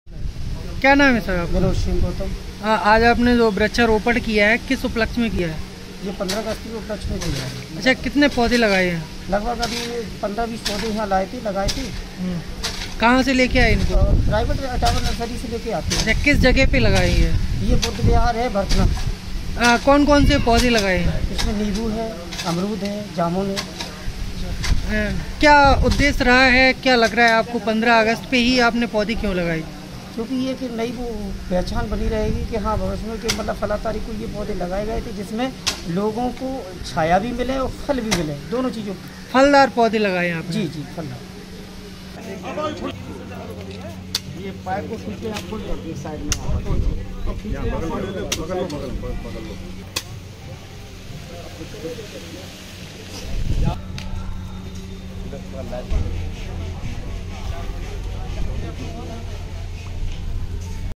यहां इतने पौधें लगाकर मनाई गई आज़ादी
MSS न्यूज

रिपोर्ट - विष्णु राठौर पूर्वी यूपी प्रभारी

इटावा यूपी

Join & Support MSS NEWS 24X7 News Channel -